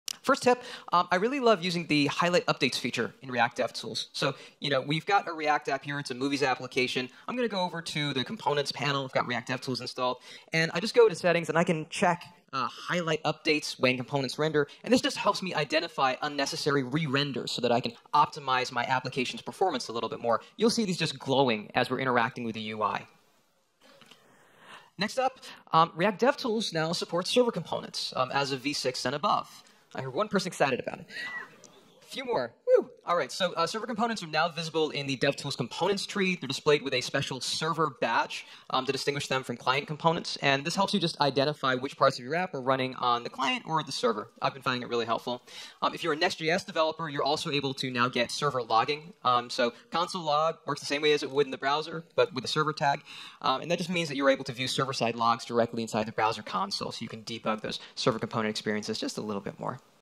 New React Devtools Features!
In this talk, Addy Osmani, talks about new features in react DevTools.